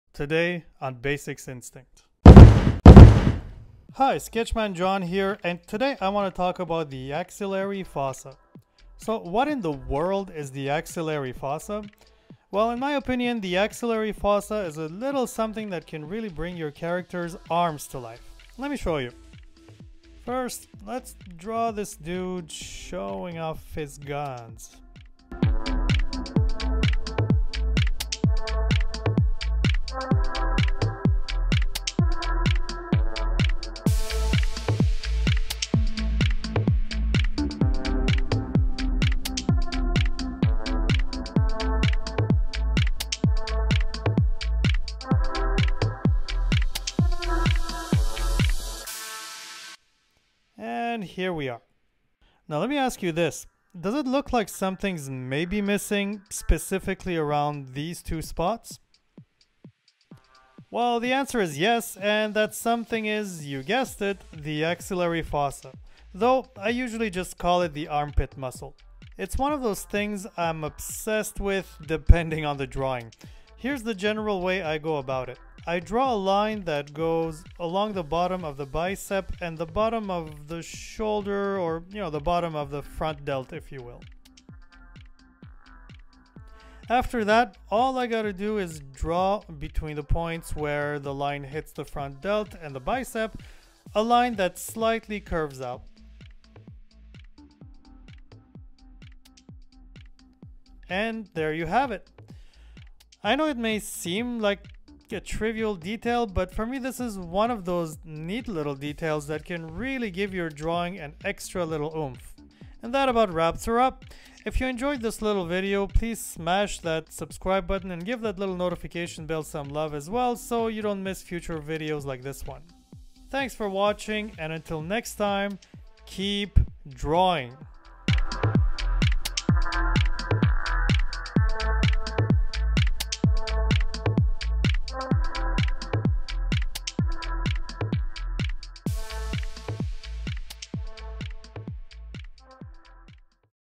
Bring Arms to Life - The Axillary Fossa - Basics Instinct #3
Sometimes, the devil's in the details. This video goes over the axillary fossa, the magic little line that makes the difference between a character with good arms and one with awesome looking arms!

Yes, in the video I call it "the armpit muscle", and yes, technically it doesn't seem to be a muscle, but I find it makes the arm look extra jacked, so I think I'll keep calling it a muscle. Sorry!

Today, on Basics Instincts...

Hi, Sketchman John here, and today I want to talk about the axillary fossa.

So, what the world the axillary fossa? Well, in my opinion, the axillary fossa is a little something that can really bring your character’s arms to life. Let me show you.

First, let’s draw this dude, showing off his guns.

Aaaand here we are.

Now let me ask you this: does it look like something’s maybe missing, specifically around these two spots?

Well, the answer is yes, and that something is, you guessed it, the axillary fossa, though I usually just call it the armpit muscle. It’s one of those things I’m obsessed with, depending on the drawing.

Here’s the general way I go about it. I draw a line that goes along the bottom of the bicep and the bottom of the shoulder, the bottom of the front delt, if you will.

After that, all I gotta do is draw, between the points where that line hits the front delt and the bicep, I line that slightly curves out.

I know it may seem trivial, but for me this is one of those neat little details that can really give  your drawing an extra little oomph.